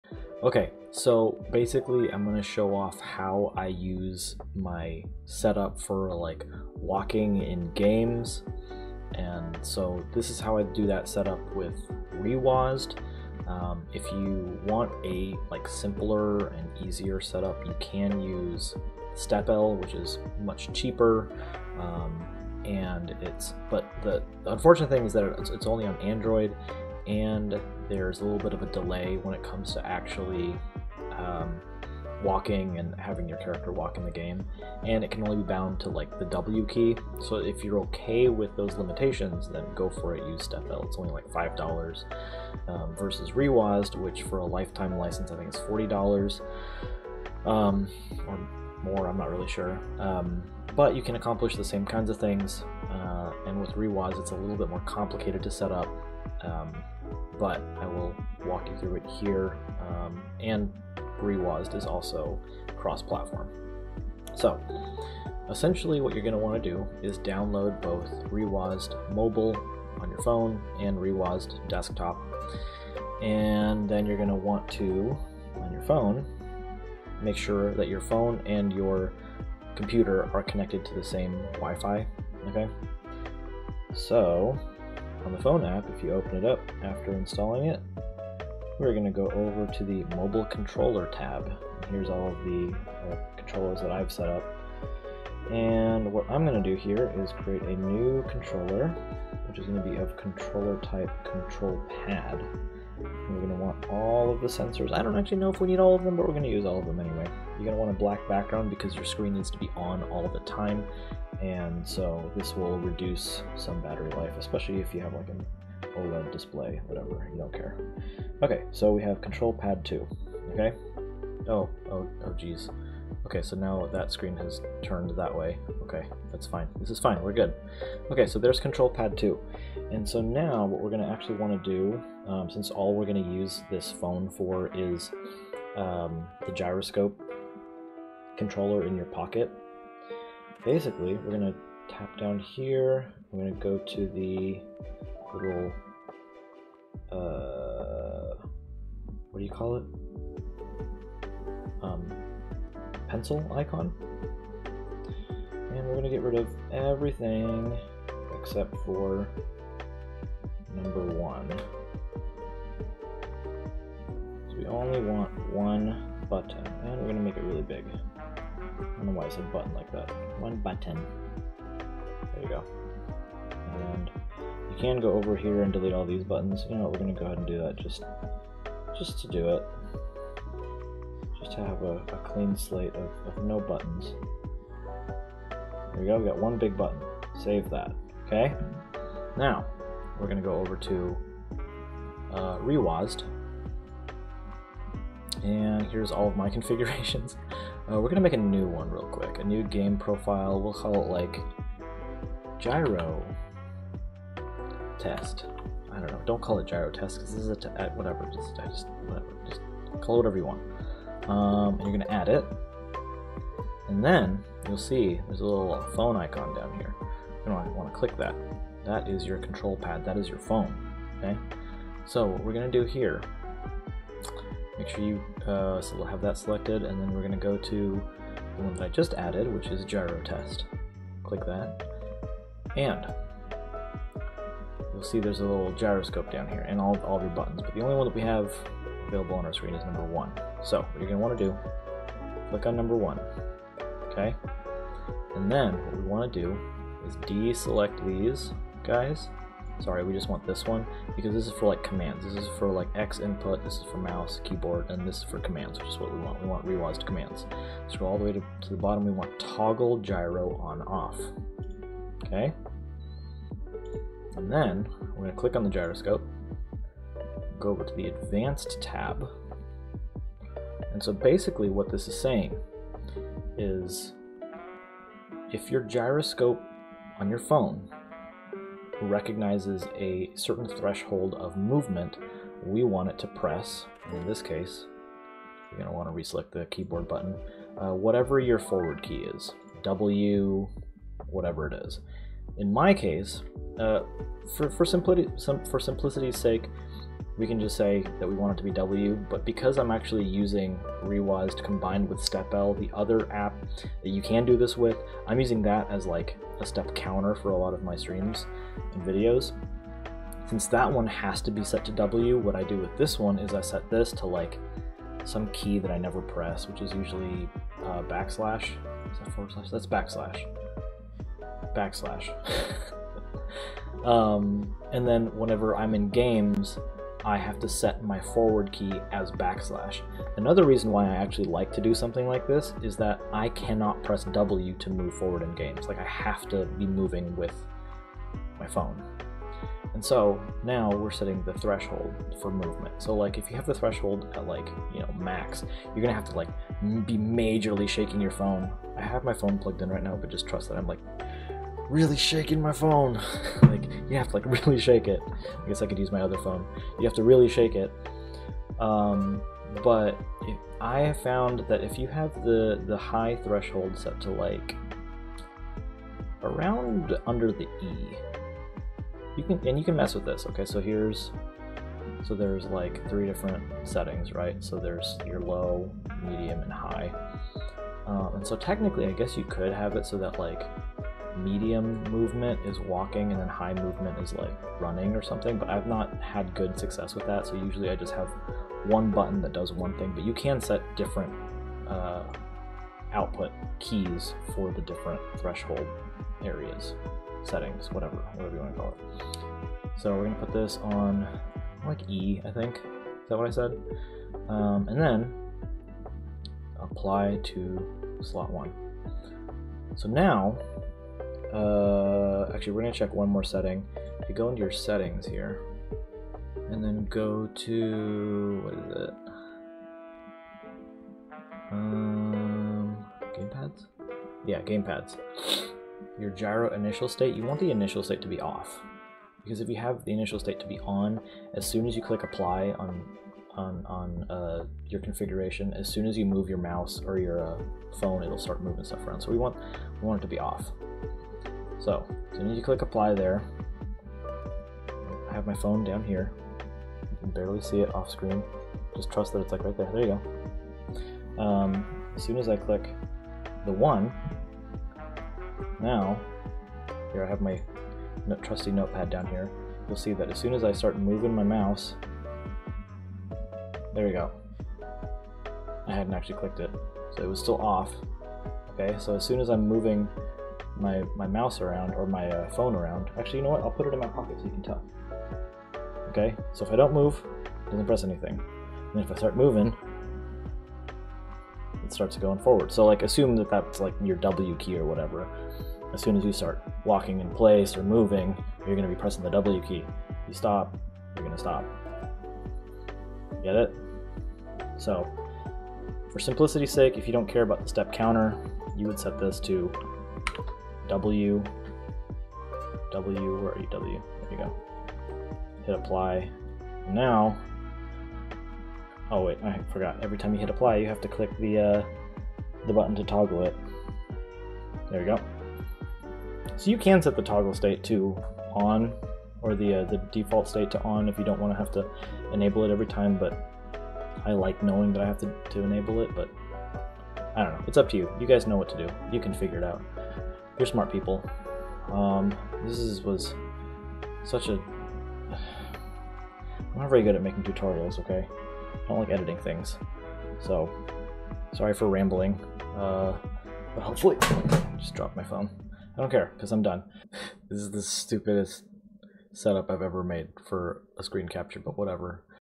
How to Walk in Games with reWASD! (iOS/Android)
reWASD if you want to buy a lifetime license!

Also, here is my video about how to set this up with Stepl, which is much cheaper and easier to set up, but it is Android only, only lets you press W in games, and has a slight delay with walking. Still a great alternative